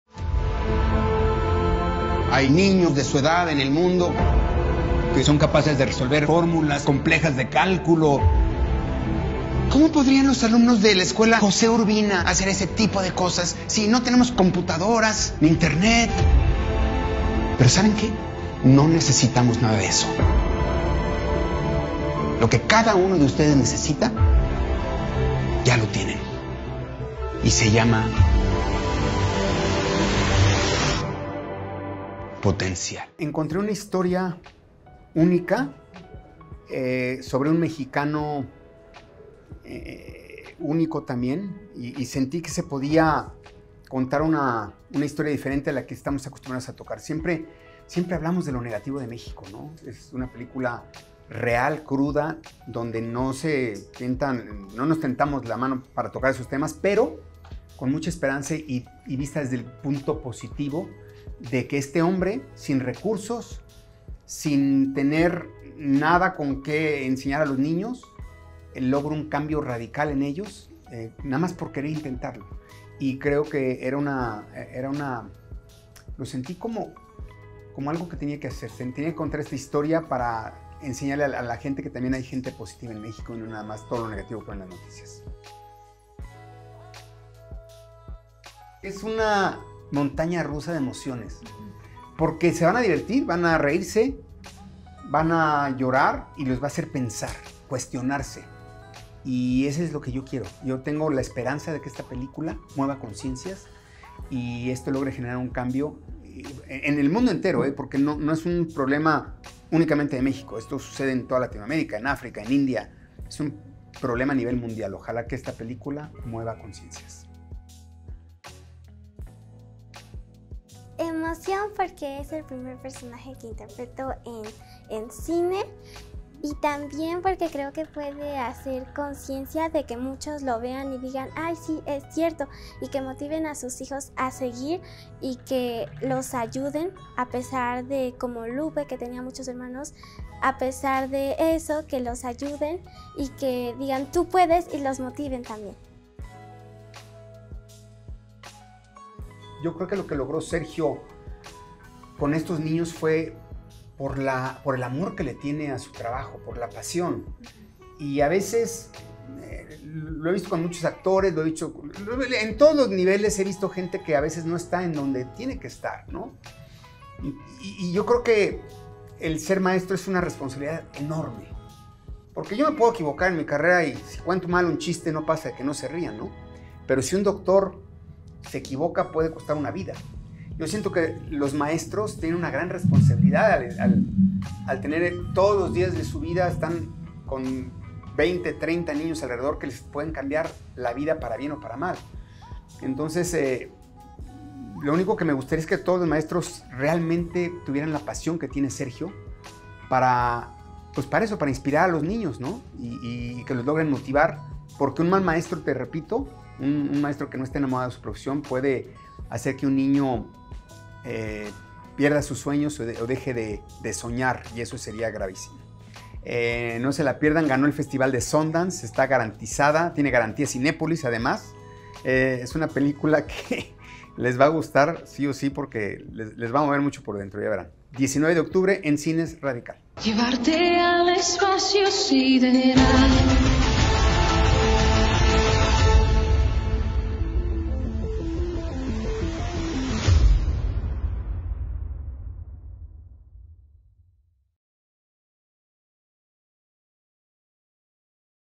Radical: Platicamos con Eugenio Derbez sobre su nueva película
En esta entrevista Eugenio Derbez nos platicó de su nueva película. Radical es un filme que retrata la historia cruda de un profesor que lucha por darles la mejor educación a sus alumnos. Sin embargo, también es una peli que nos va llenar de esperanza. ¡Anímense a verla!

🌶️ Acerca de Chilango:
Te decimos qué hacer en la Ciudad de México: comida, antros, bares, música, cine, cartelera teatral y todas las noticias importantes.